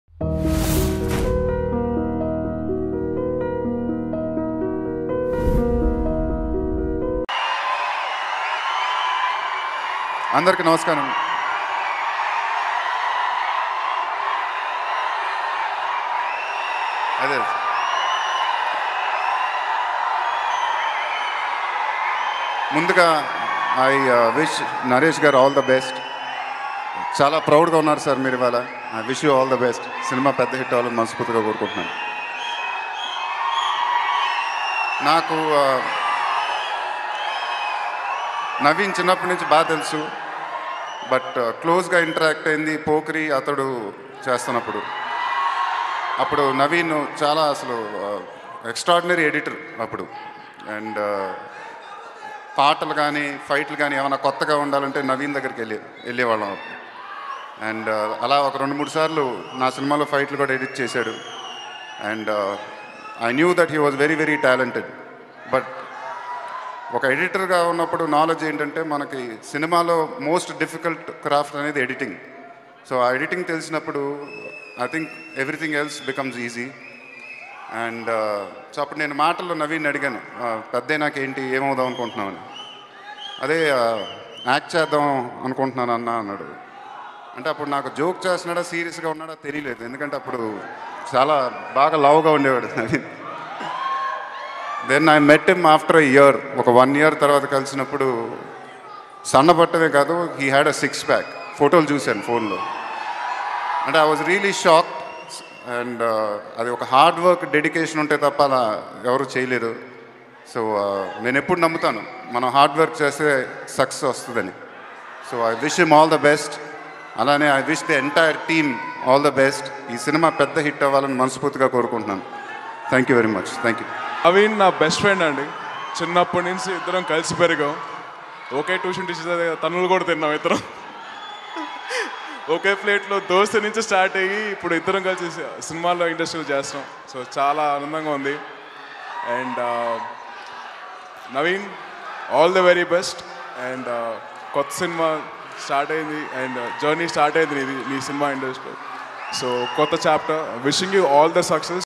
Mahesh Babu Speech @ Nandini Nursing Home Audio Launch || Nawin Vijay Krishna || Nitya || Sravya
Watch Mahesh Babu Speech at Nandini Nursing home Audio Launch.  Starring Nawin Vijay Krishna, Nithya and Shravya. Directed by PV Giri, Producers by Radha Kishore and Bikshamayya. Music composed by Achu Rajamani and Shekhar Chandra.